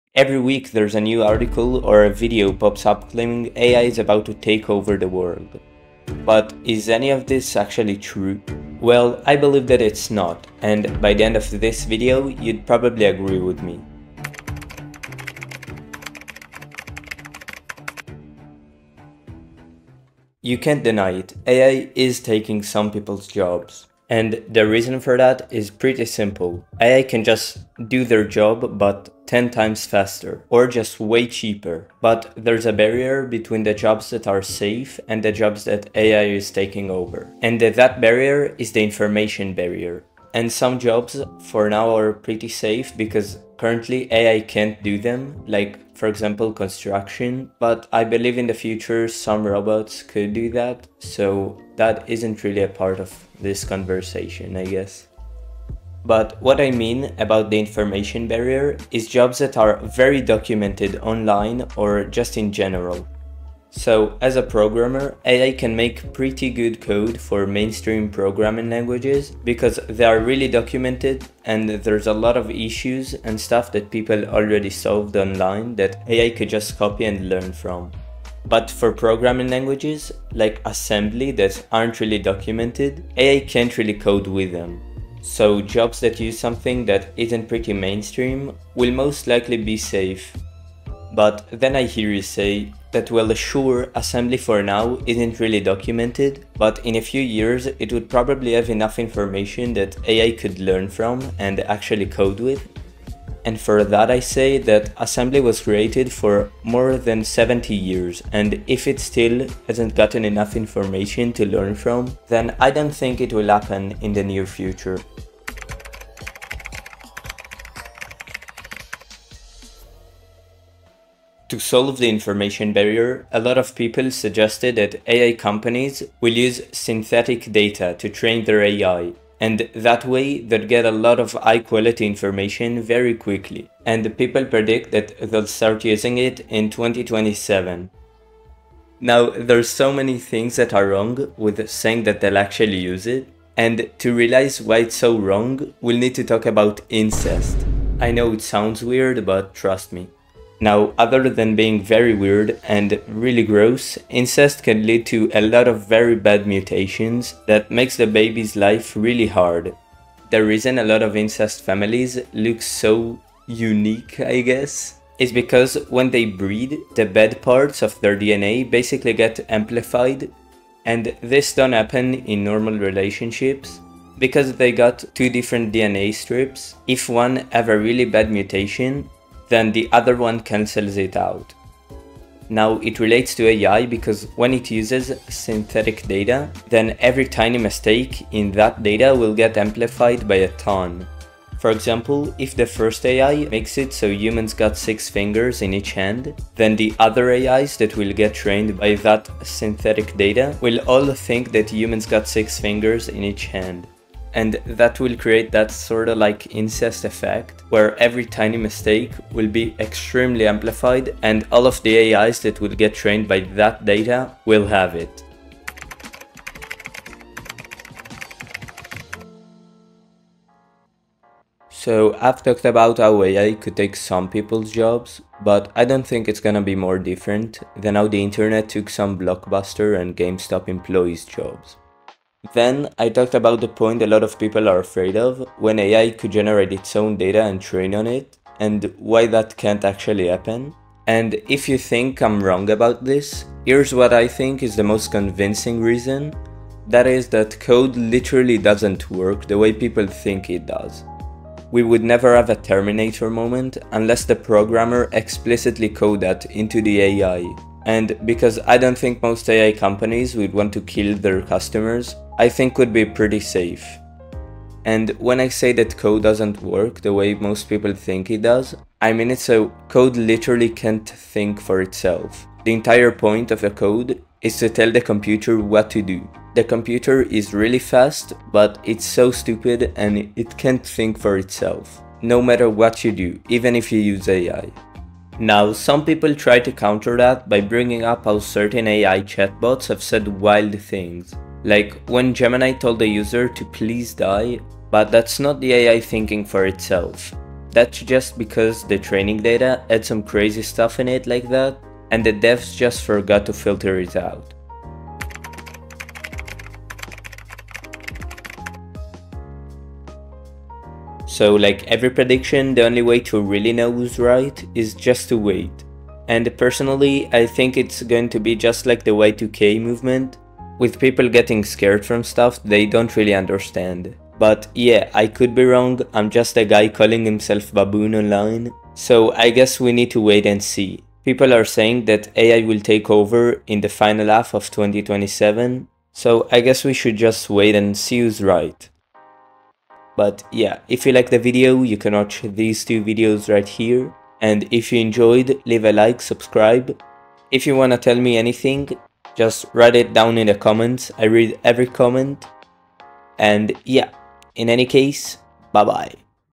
Why AI Won’t Take Over the World
So in the video, I kept referring to how many people are saying that AI will reach a point where AI researchers become superhuman.

It’s an interesting read, but definitely don’t take what it says there as true predictions.